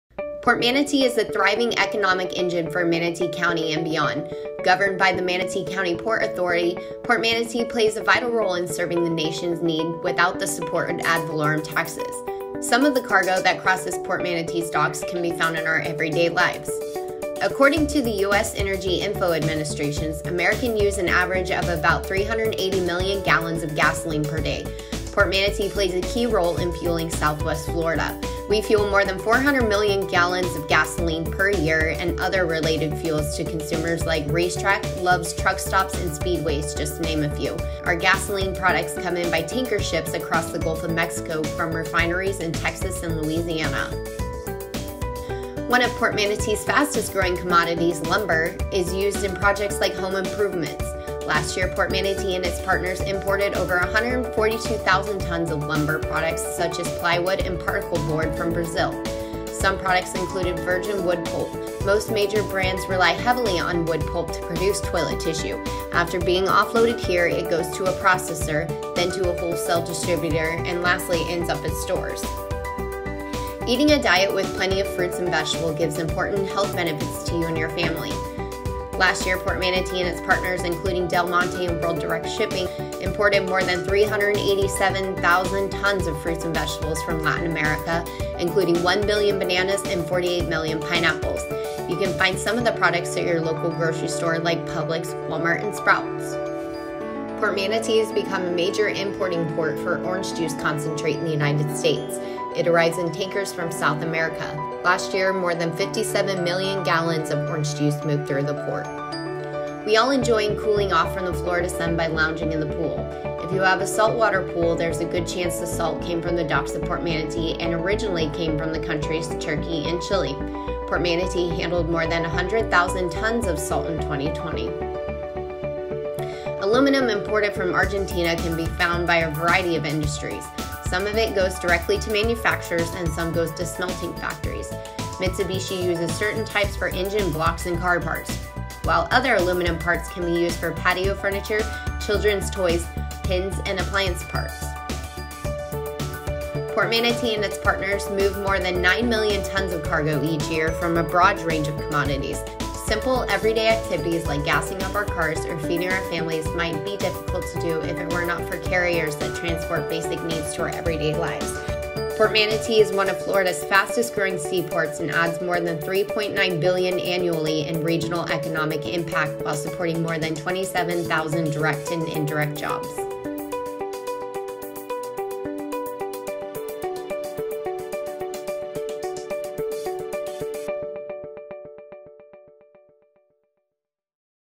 Port Manatee Commodities
International trade is a critical component to our nation’s economy and Port Manatee plays a vital role in serving the nation’s needs. Some of the cargo that crosses Port Manatee’s docks can be found in our everyday needs. So where are the goods coming from and where are they going?